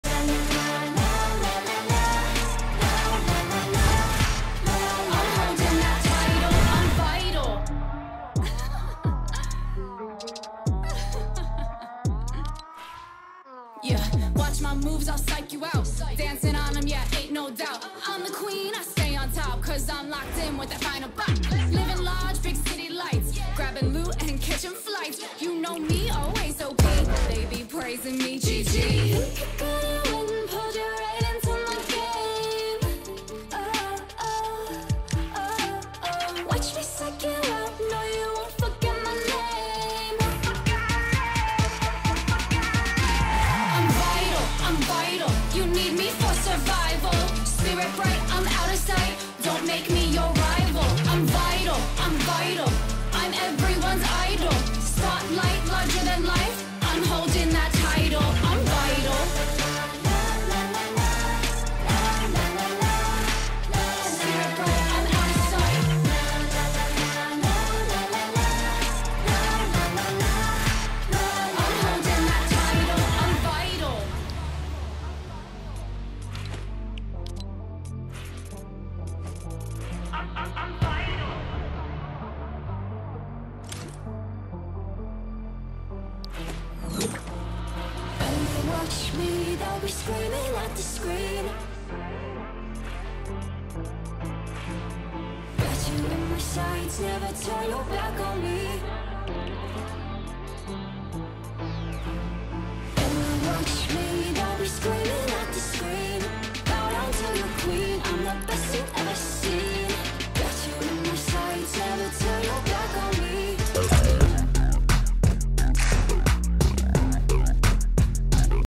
I GOT GIFTED THE SWEATIEST SKIN FROM NINJA!!! 😮😮
Sorry for the inconsistent upload schedule and I know I missed creative 2.0 but I am planning on making a video on it soon! Anyways, enjoy the video!